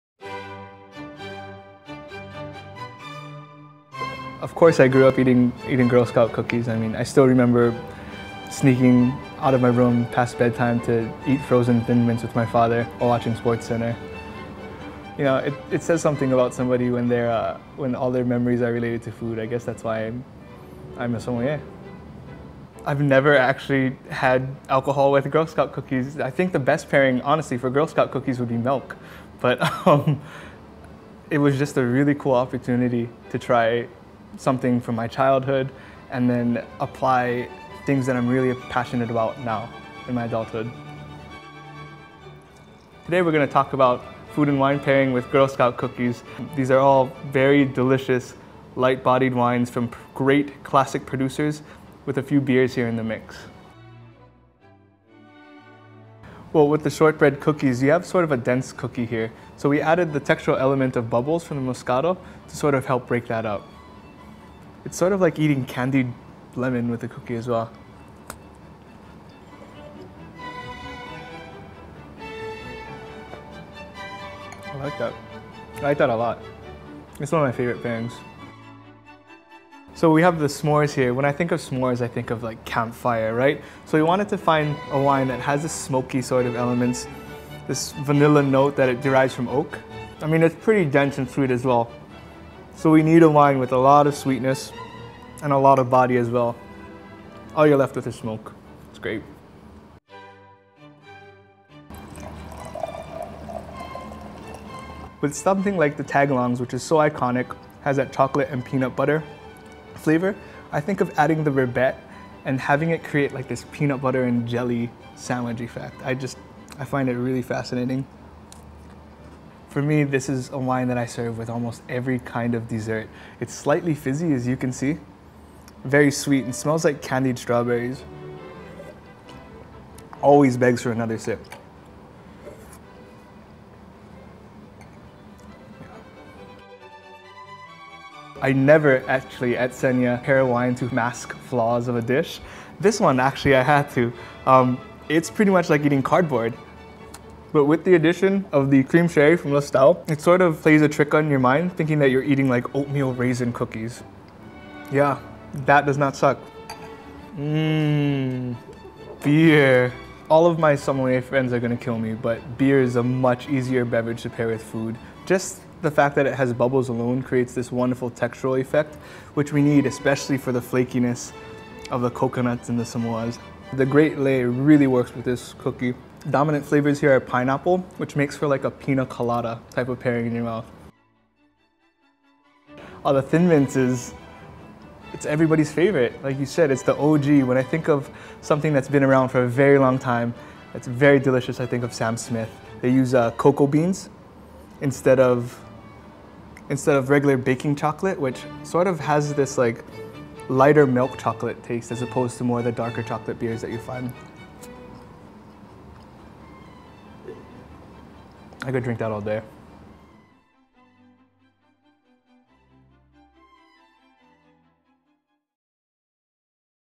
A Hawai‘i Sommelier Pairs Wine and Girl Scout Cookies
Senia wine director Chris Ramelb pairs six Girl Scout cookies with wine.